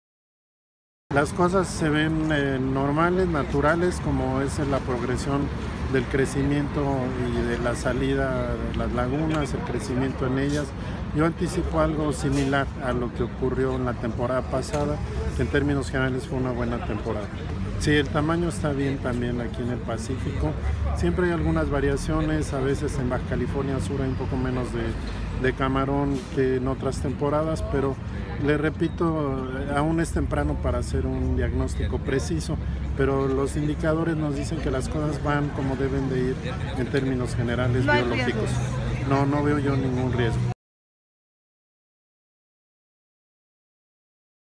No hay riesgos en el desarrollo del camarón en altamar: INAPESCA
Pablo Arenas Fuentes, Director General del Instituto Nacional de la Pesca, aseguró que de acuerdo a los últimos estudios y muestras tomadas por el INAPESCA, el crustáceo en altamar se mantienen en buenos niveles de crecimiento y reproducción.
Descartó la posibilidad de que esté en riesgo la temporada camaronera en el pacífico por cuestiones de reproducción del crustáceo.